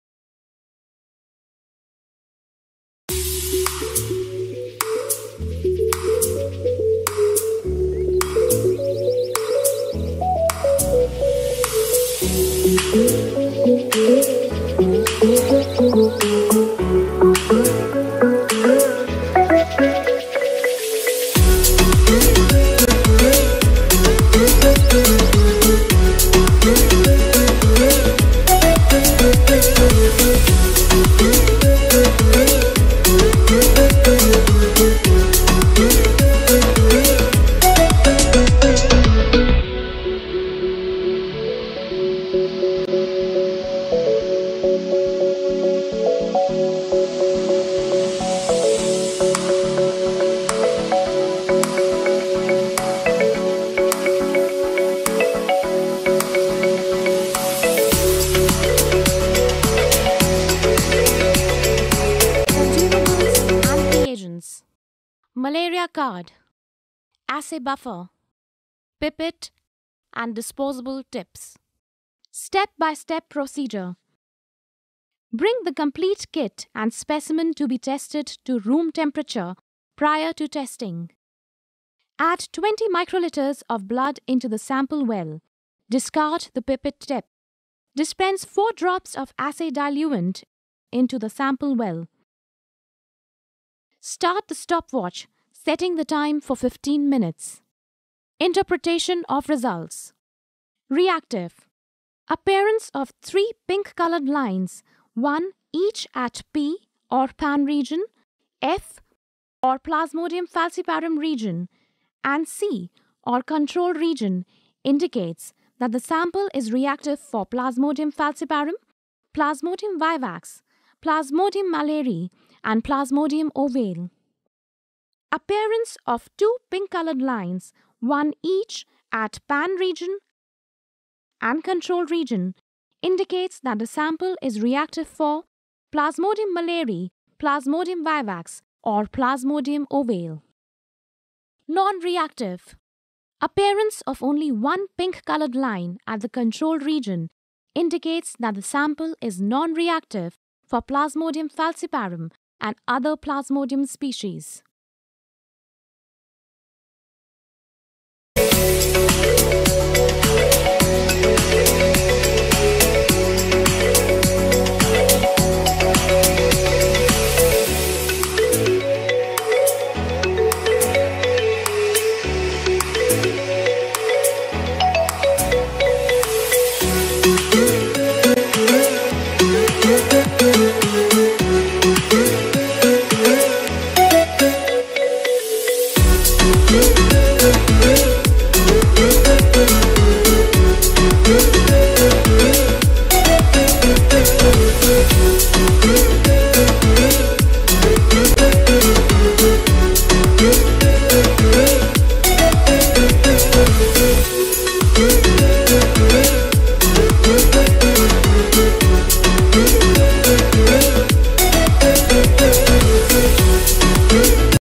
Malaria P.f. /Pan Rapid Test Cassette | MP ICT Rapid Test | Malaria Antibody Detection Test
The objective of this video is to appraise the viewer about:
What is the ICT Malaria test?
Does this test have other names?
What is the purpose of this test?
Why do I need this test?
Diagnostic test (Malaria P.f. /Pan Rapid Test Cassette)
What do my test results mean?
What other tests might I have along with this test?
What causes malaria?
Can malaria kill you?